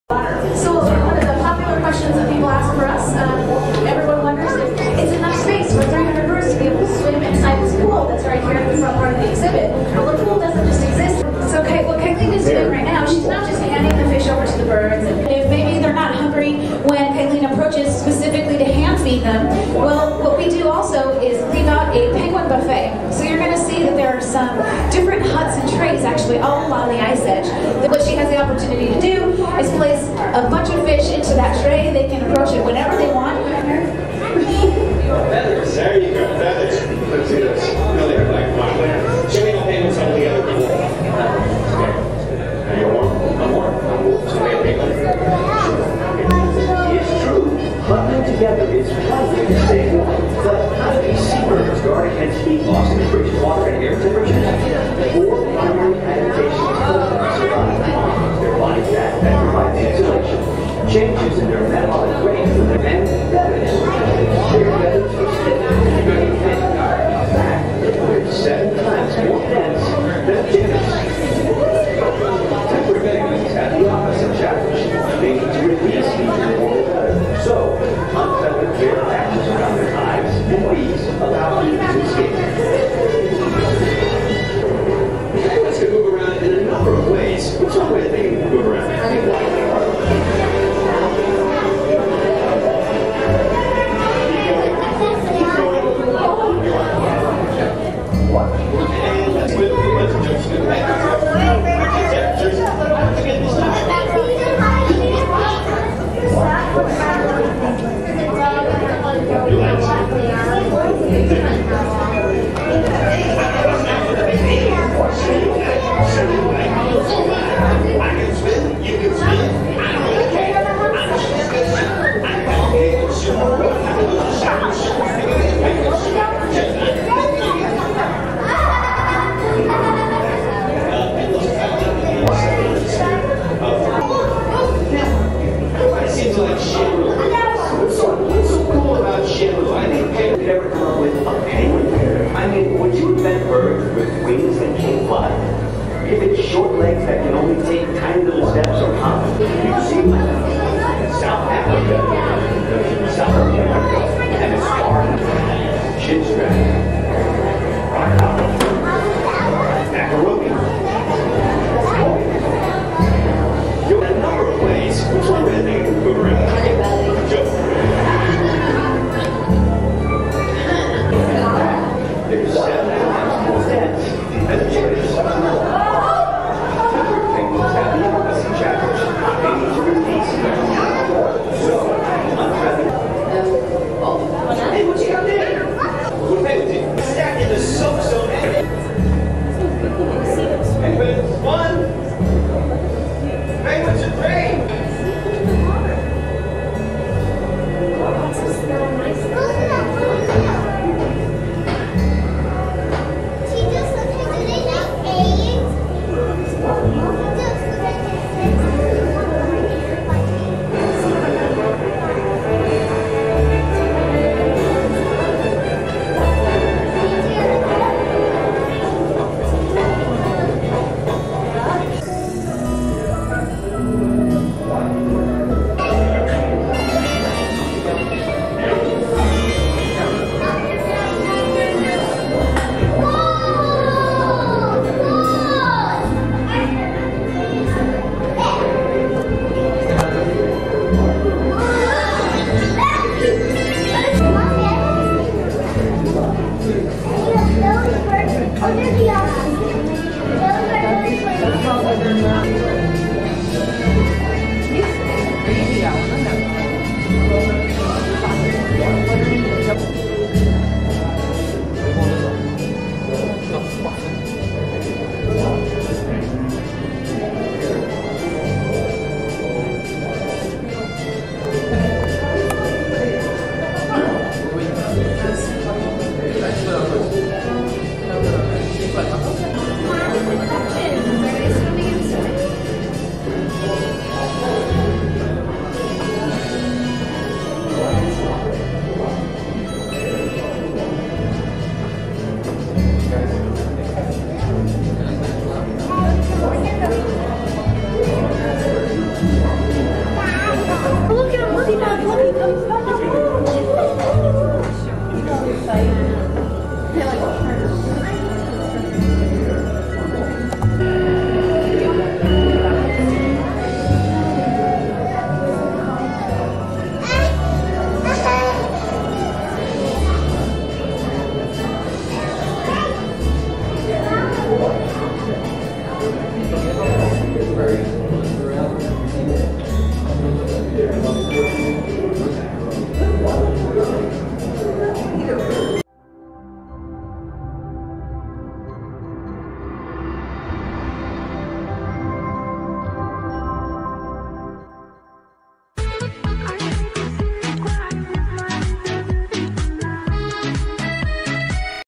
Video Clips - See Penguin & Tufted puffin on big Aquarium SeaWorld's
SeaWorld is a United States theme park chain with headquarters in Orlando, Florida. It is a proprietor of marine mammal parks, oceanariums, animal theme parks, and rehabilitation centers owned by SeaWorld Entertainment.